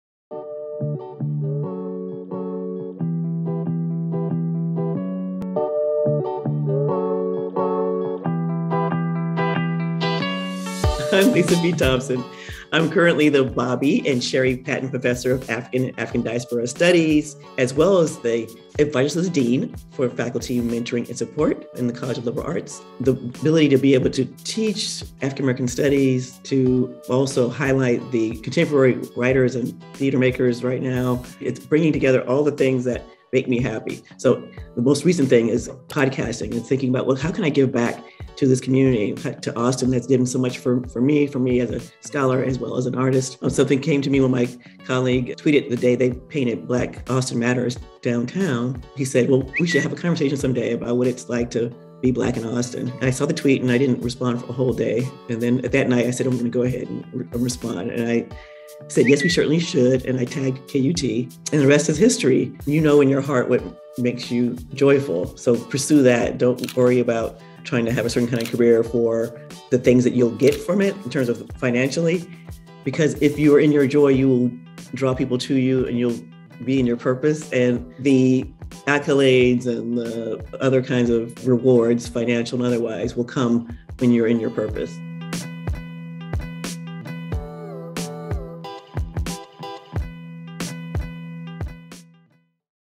People of Liberal Arts: Dr. Lisa B. Thompson
Dr. Lisa B. Thompson, professor of African and African Diaspora Studies, tells the origin story of the "Black Austin Matters" podcast and gives candid advice for students in the College of Liberal Arts. To read more about Dr. Thompson and check out more People of Liberal Arts articles,